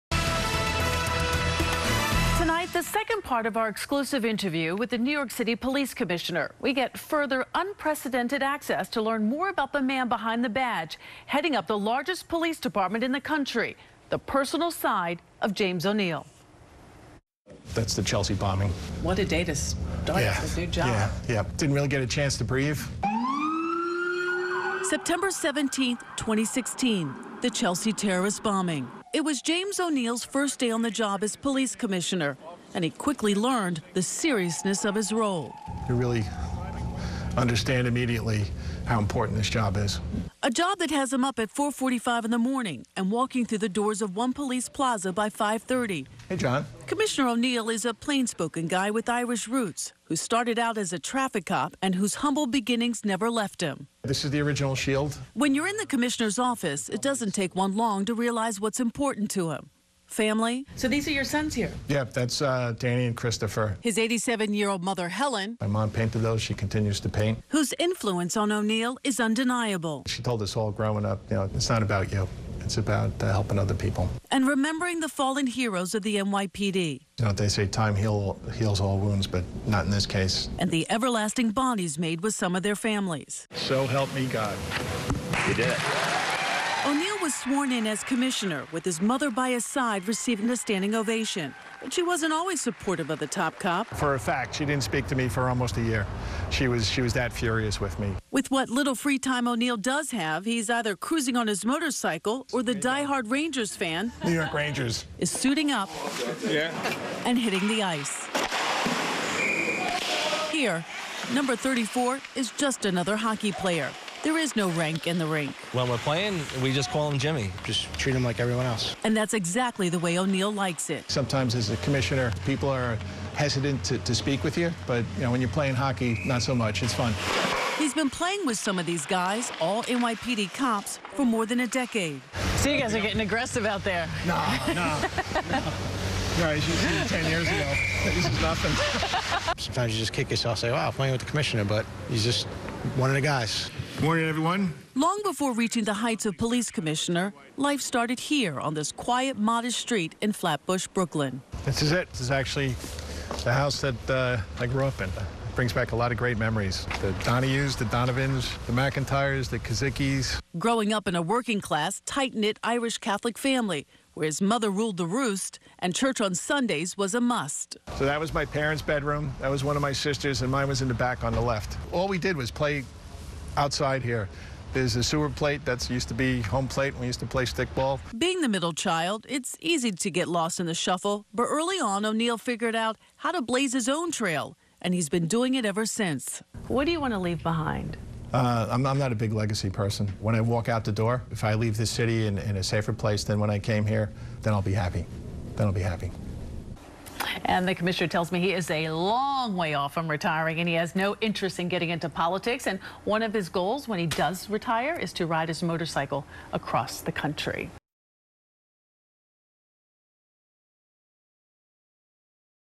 Meet James O'Neill: Family man, hockey fan, and NYPD's top cop
September 17, 2016: A bomb exploded in New York City's Chelsea neighborhood, causing chaos and confusion among city residents. This was Police Commissioner James O'Neill's first day on the job.

"Didn't really get a chance to breathe first full day on the job," he told Eyewitness News Anchor Sade Bade in his One Police Plaza office.

He quickly learned the seriousness of leading the United States' largest municipal police department, comprised of 77 patrol precincts, 36,000 officers and 19,000 civilian employees, according to the NYPD.

NEW HERE? –